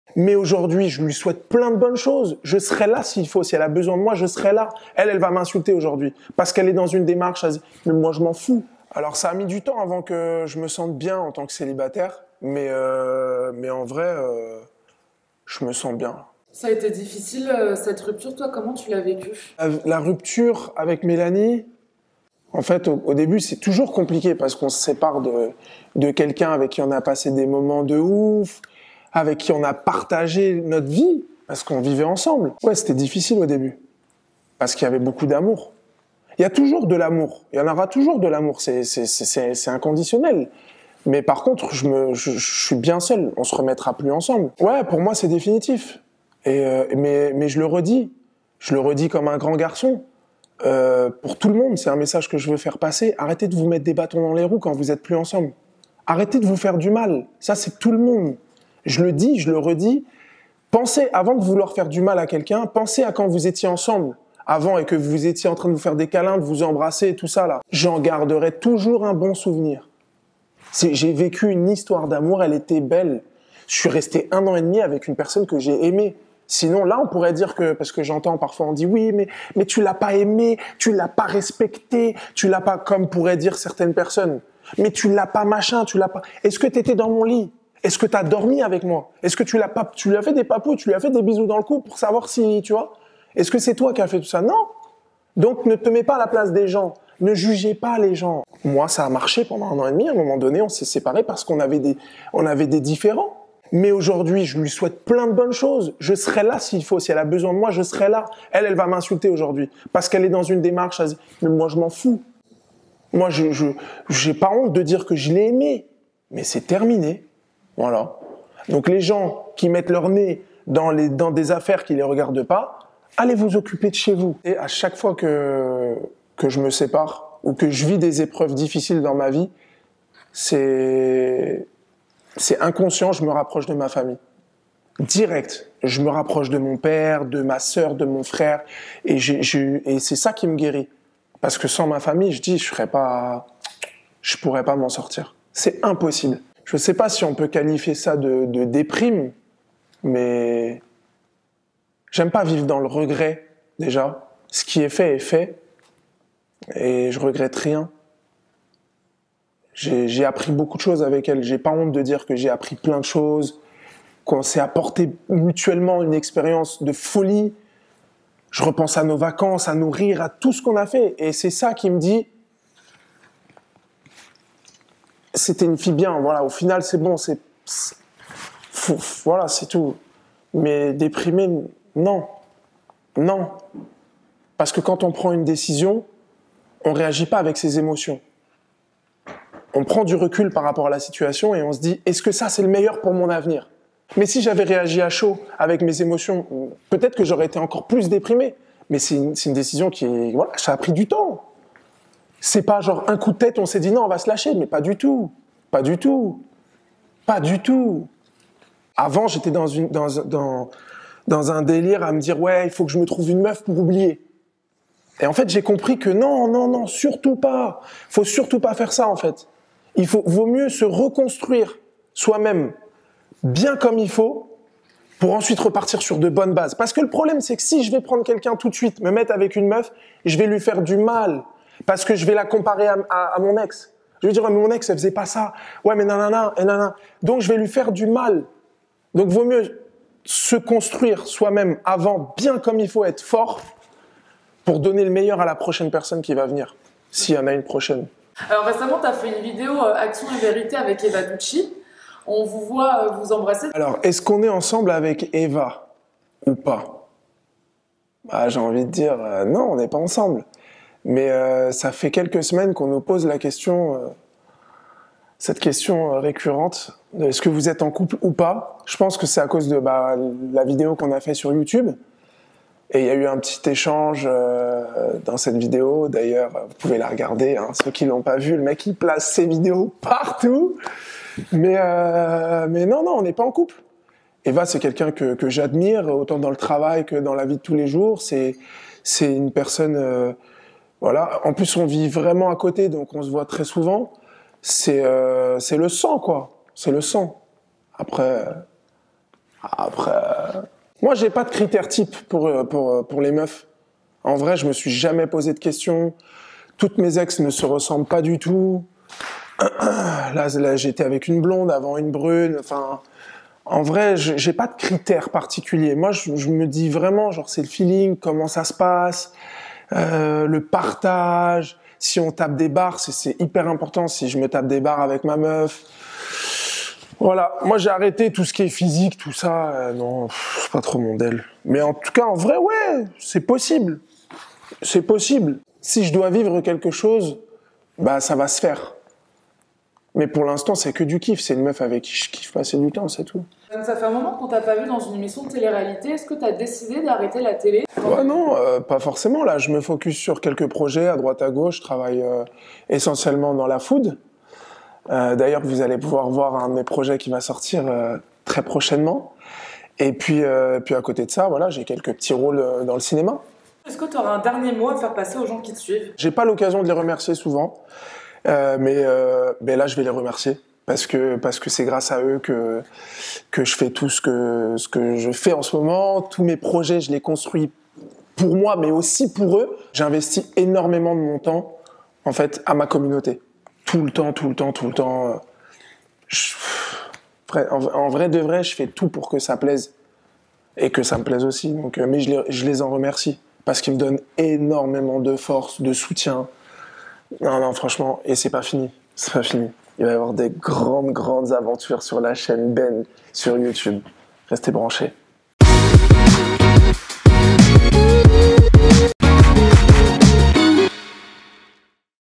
ROMAIN BENN : POUR LA PREMIÈRE FOIS IL SE CONFIE À COEUR OUVERT SUR SA RUPTURE AVEC MÉLANIGHT ! 😢💔
🌍 DANS LA GOSSIP SPHÈRE AUJOURD’HUI 🌍

✨Séparé depuis plusieurs mois de Mélanight, Romain se confie sur sa rupture. Si pour lui, cette épreuve n’a pas été facile, il a su tourner la page grâce à sa famille. Aujourd’hui, il souhaite le meilleur à son ex même si du côté de Mélanight ça ne serait pas réciproque…

✨COUP DE GUEULE - Romain adresse un message à tous ceux qui ont jugé sa manière d’être avec Mélanight. Il explique que personne n’a a jugé son couple.

✨Il répond également à cette interrogation qu’on se pose tous : Est-il en couple avec Eva Ducci ? Il répond en toute sincérité !

✨ Si sa chaîne Youtube fait un carton, Romain ne s’arrête pas là ! En effet, plusieurs projets l’attendent dans le domaine du cinéma. Affaire à suivre…

🚀 Réagis en commentaire à cette vidéo !